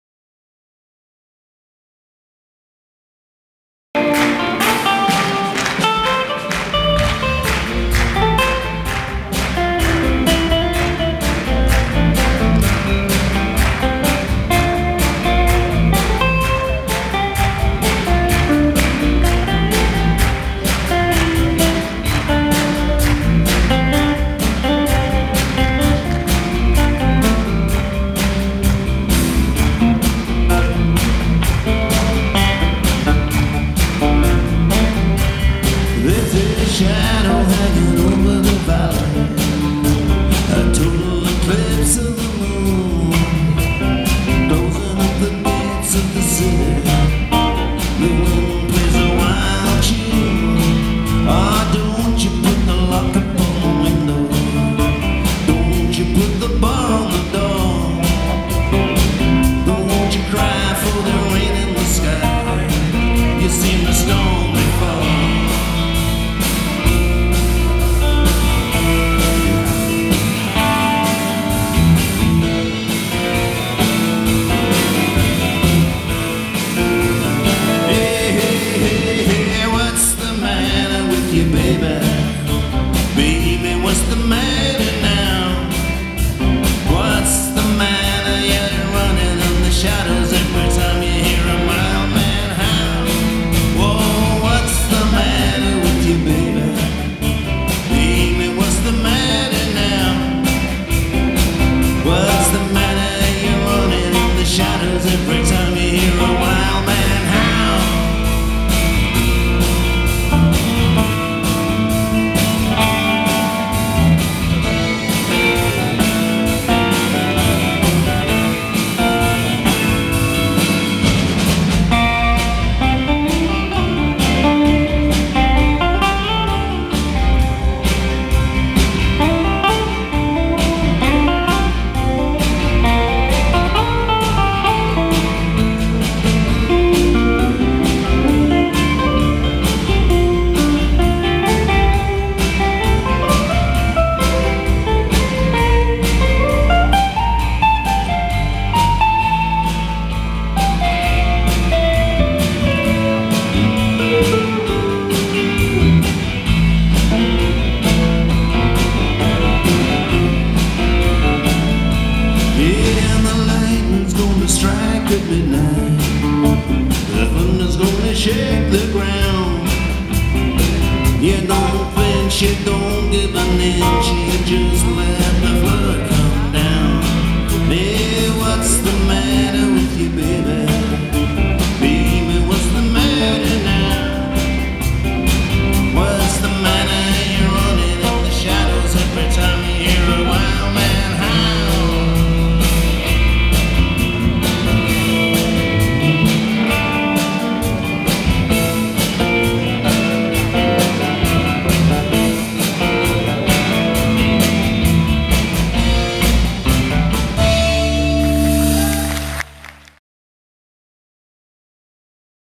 DAVID KNOPFLER, EX DIRE STRAITS CAUTIVA AL PÚBLICO DE L'AUDITORI DE LA MEDITERRÀNIA
El pasado sábado 19 de febrero en l'Auditori de la Mediterrània  actuó una leyenda del rock David Knopfler, ex Dire Straits, que incluyó La Nucía dentro de su gira Electric Guitar Tour 2011 que le llevará por las principales capitales de España. Esta actuación rozó el lleno de las butacas del auditorio nuciero.